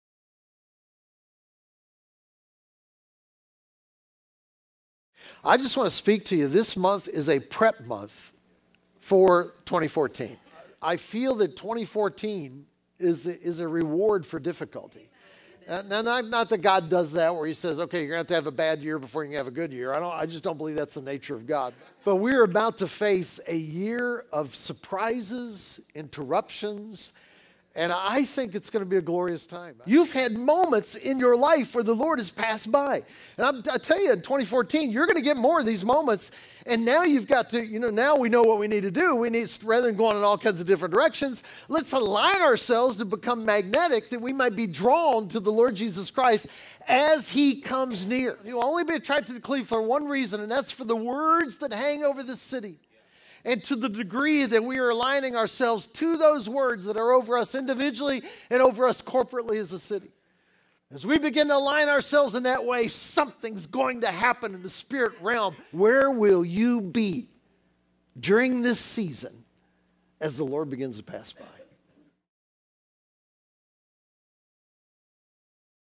Bethel Cleveland December 8, 2013: Steve Witt
Segments from Senior Leader, Steve Witt's message, "The Advent Adventure."